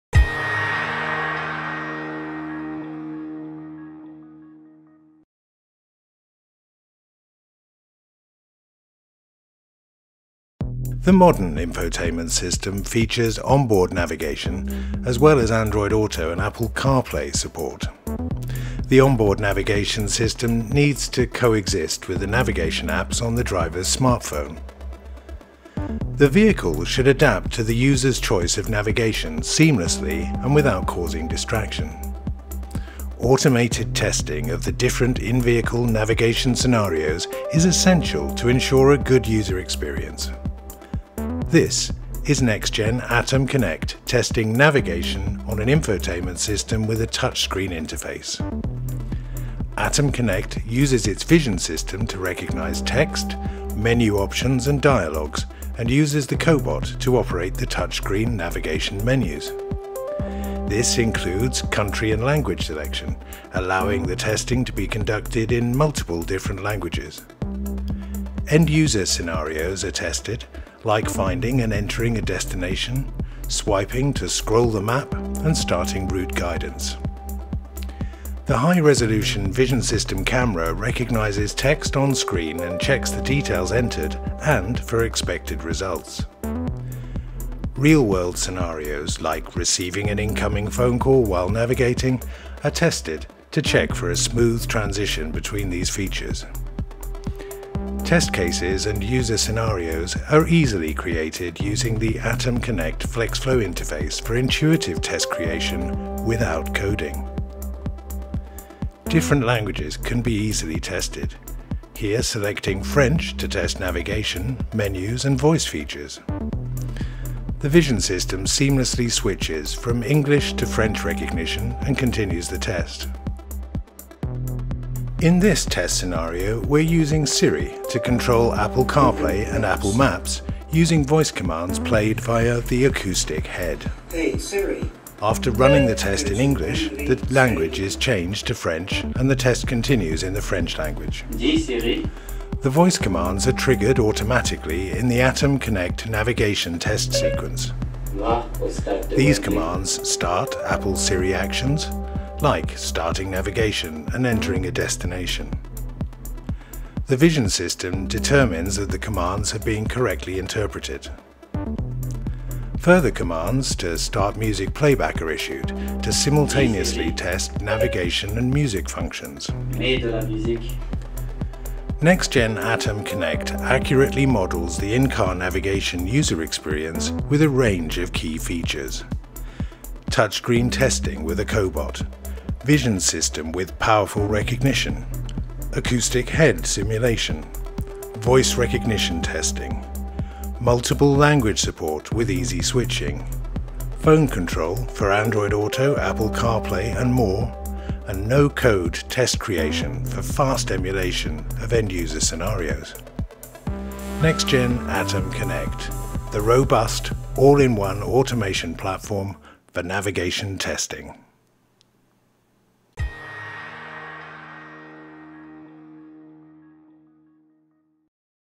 ATAM Connect automated infotainment navigation testing with cobot, acoustic head and vision system
Exploring infotainment Navigation system testing with Nextgen ATAM Connect - fast and intuitive infotainment touchscreen navigation testing with a collaborative robot and vision system.

In this video, we explore using automated testing for in-car navigation systems to evaluate all features and user functions of the modern infotainment system in multiple regions and languages.

ATAM Connect with a vision system, Cobot automation and Acoustic head provides an all-in-one automated solution integrating visual recognition, infotainment touchscreen testing, voice recognition and mobile phone control, allowing all features and user functions to be automated for advanced testing. ATAM Connect FlexFlow's unique codeless test creation allows for easy test creation of any end-user scenario without specialised coding skills.

ATAM Connect vision system and collaborative robot swiftly adapt to different user interfaces and functions, handling both the built-in infotainment user interface and Android Auto and Apple CarPlay interfaces. This allows the full scope of user experience to be evaluated.

The ATAM Connect cobot functionality allows for control of physical buttons as well as touch screens and can handle analogue rotary controls. The Acoustic head simulator supports voice recognition system testing in multiple languages.

The cobot includes safety mechanisms such as force feedback, collision control sensors and light curtains to work collaboratively and safely with human engineers, and is fully ISO compliant for deployment.